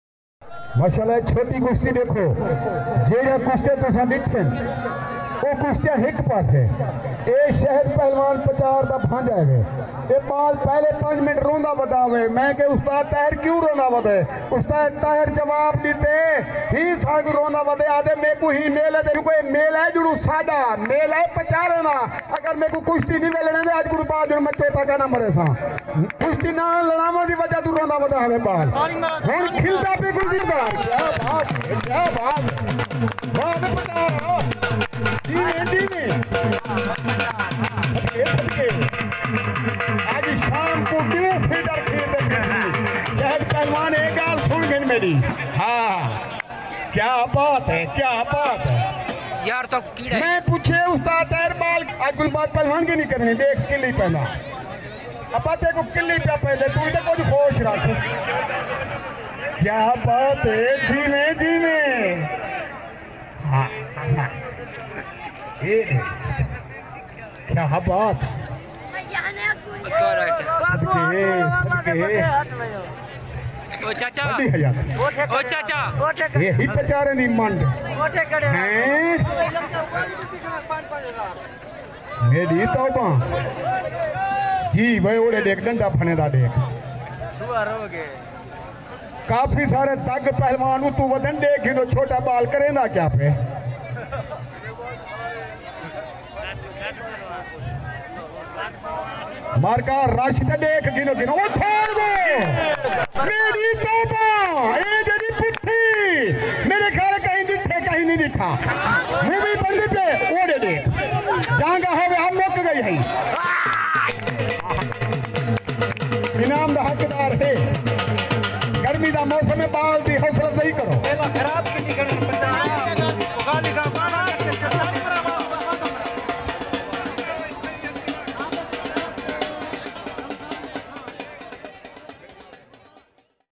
چھوٹے بچو کی کشتی بڑے پہلوانوں کو پیچھے چھوڑ دیا bachu ki kushti dekh kr pehlwan daad dene aaa agye
دوستو آج چھوٹے بچو نے کمال کردیا بڑے بڑے پہلوان داد دینے کھاڑے میں آگئے
آگر ہماری وڈیو آچھی لگے تو ضرور سبسکرائیب کرنا